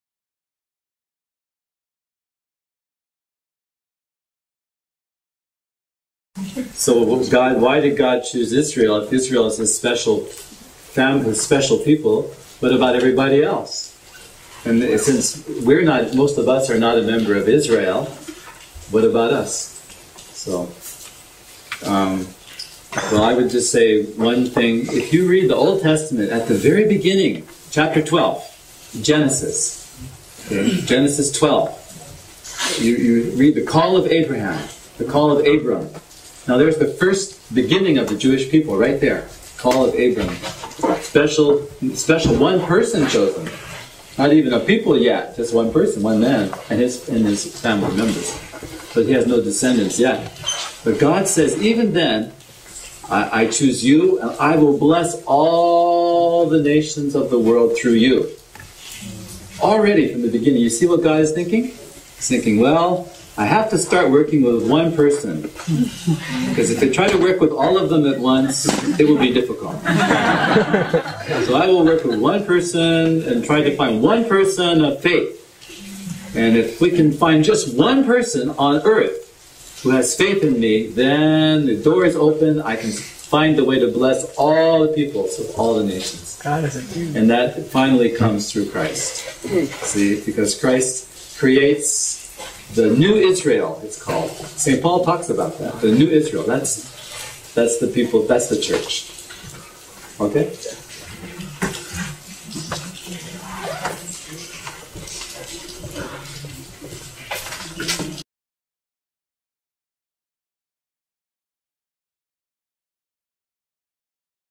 Madonna House Summer Program: Why Did God Choose Israel
The Madonna House directors answering a question on why God chose Israel as his chosen people.